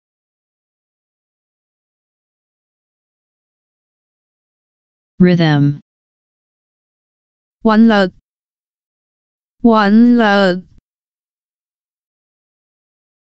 "RHYTHM" in Cantonese (韻律) - Flashcard
[wan6 leut6] "RHYTHM" in Cantonese (韻律) - Flashcard. Additional information about the word: cadence,rhythm,rhyme scheme,meter (in verse).